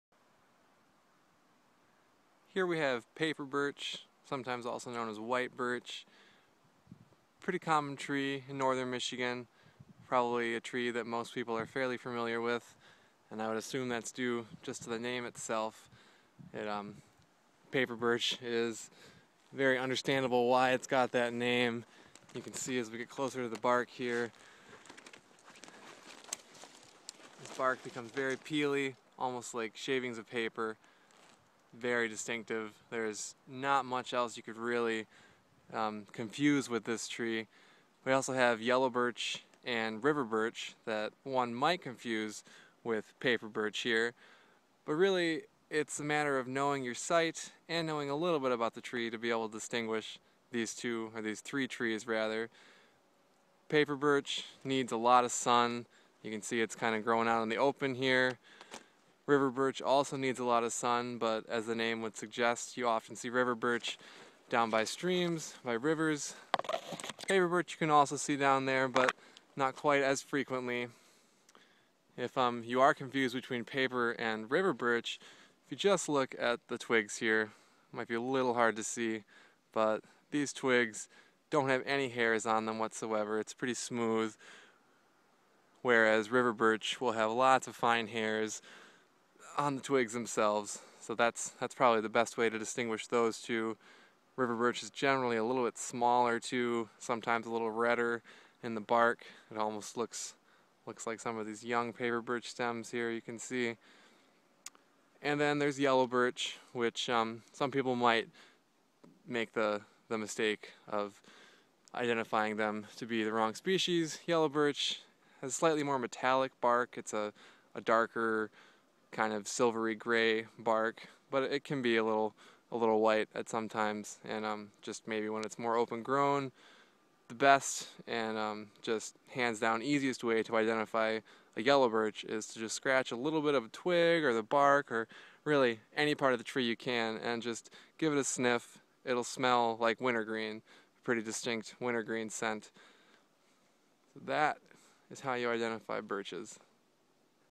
Paper Birch
The paper birch is a early successional tree with important for wildlife and timber values alike. It's pretty easy to identify, but a couple quick tricks will help you easily distinguish it from the other birches